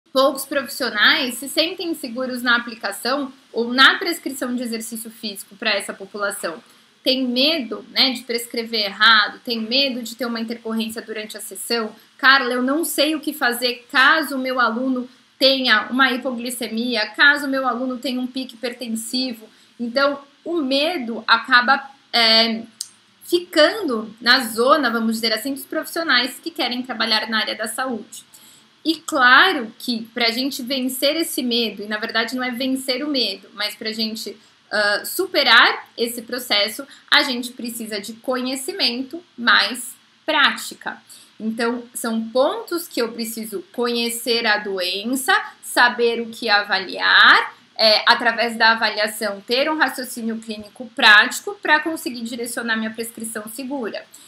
O medo de impede de trabalhar na área da saúde
O medo paralisa e a insegurança pode colocar a saúde do seu aluno em risco.